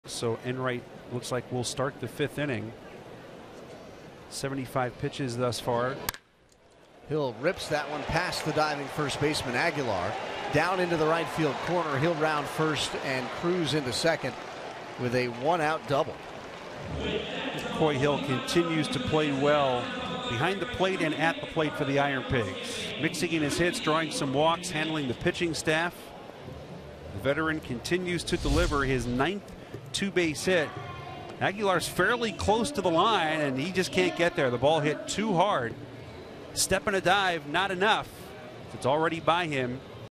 Lehigh Valley's Hill hits one-out double
6/21/14: Lehigh Valley's Koyie Hill hits a one-out double in the fourth inning of a 14-2 loss to Columbus